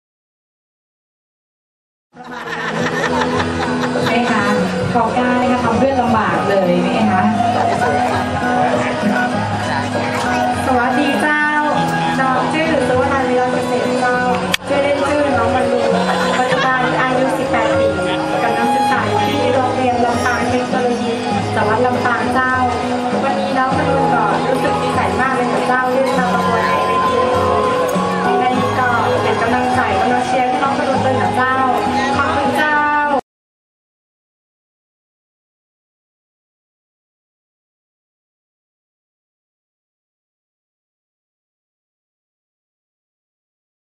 ミス・クイーン2009(22)(MissQueen2009)ニューハーフ美人コンテスト・タイ
タイ・チェンマイで開催されたニューハーフ美人コンテストの第一審査の自己紹介の映像です。It is an image of the self introduction of the first examination of newhalf(ladyboy) the beautiful woman contest held by the Thailand Chiang Mai.